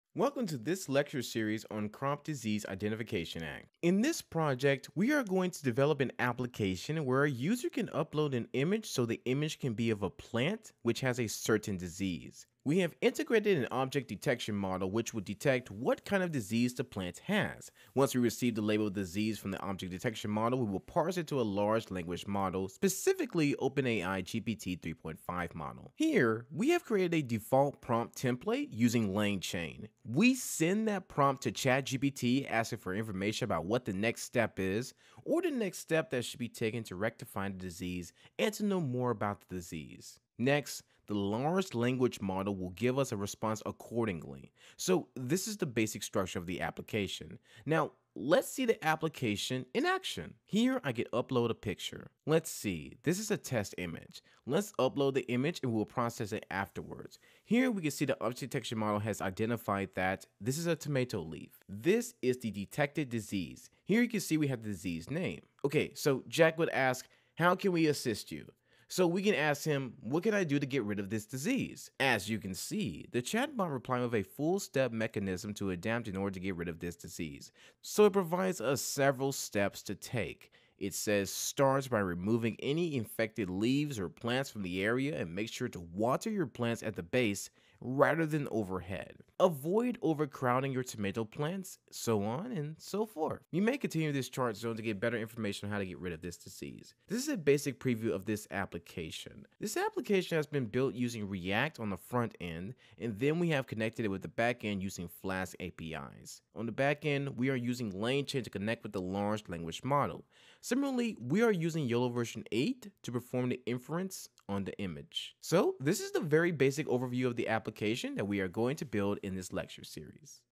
AI-Powered Crop Disease Identification: Introduction to Plant Health Solutions
App 1.1 Crop Disease Identification Introduction

Welcome to our lecture series on developing an AI-powered Plant Disease Identification Application. This project combines cutting-edge technologies like object detection and large language models to revolutionize how we manage plant health. Here's what you'll learn:

🔍 What Does This App Do?
Users can upload images of plants affected by diseases.
An integrated YOLOv8 object detection model identifies the disease type.
The detected label is sent to OpenAI's GPT-3.5 via a structured prompt, generating actionable insights.

🌟 Key Features of the App
Object Detection: Automatically identifies plant diseases from uploaded images.
Disease Insights: Leverages a language model to explain the disease and recommend steps for treatment.
Interactive Chat: Chatbot integration enables users to ask follow-up questions for tailored advice.
Tech Stack:
Frontend: Built using React.
Backend: Powered by Flask APIs and LaneChain for language model interaction.
AI Models: YOLOv8 for image inference and GPT-3.5 for generating responses.

A step-by-step demonstration of the app in action.
How the object detection model identifies plant diseases.
Insights from the chatbot, including detailed treatment steps like removing infected leaves, watering tips, and spacing guidelines.
A preview of the app’s architecture and functionality.

This application is a game-changer for farmers, gardeners, and agricultural experts. By automating disease detection and providing precise, actionable insights, it reduces the guesswork and effort in maintaining plant health.

🛠️ Learn How to Build It
Follow this lecture series to understand the full development process and gain hands-on experience with:

React for UI
Flask for backend integration
YOLOv8 for object detection
LaneChain and GPT-3.5 for advanced conversational AI

🎥 Watch now to learn how AI can transform plant disease management!